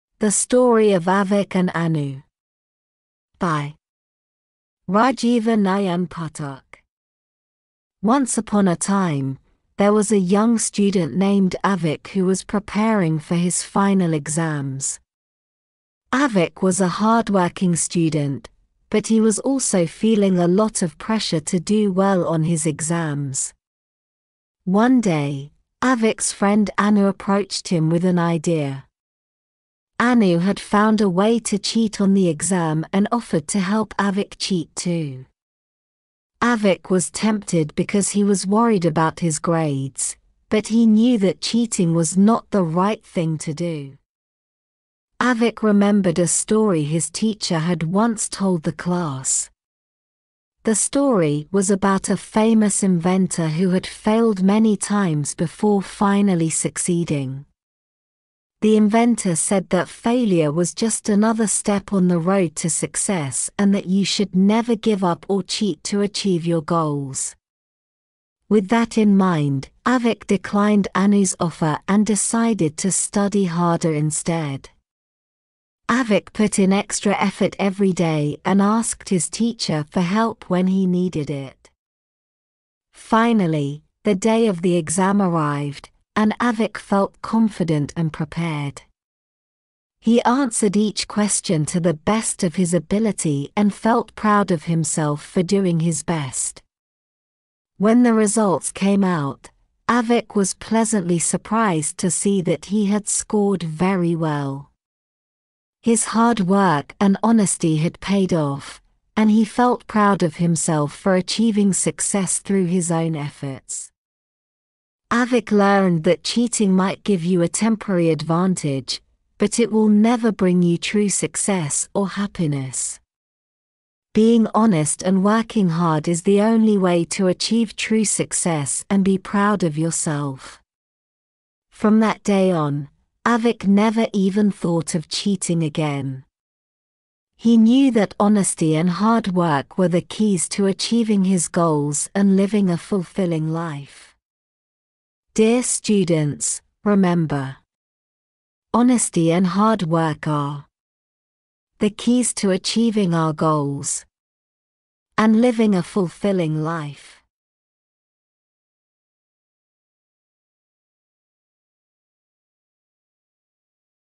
Avik and Anu
Should cheating during exams be allowed?